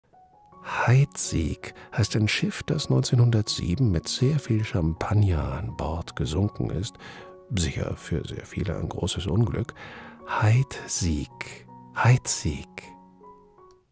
Heidsieck. Richtige Sprechweise. Korrekte Aussprache. Gutes Deutsch. Von TV-Profisprecher.
Heidsieck, ein 1907 gesunkenes Schiff. Wie spricht man das? Ich helfe bei Zweifelsfällen der deutschen Aussprache.
Erwähnung und Vermeidung häufiger Aussprachefehler.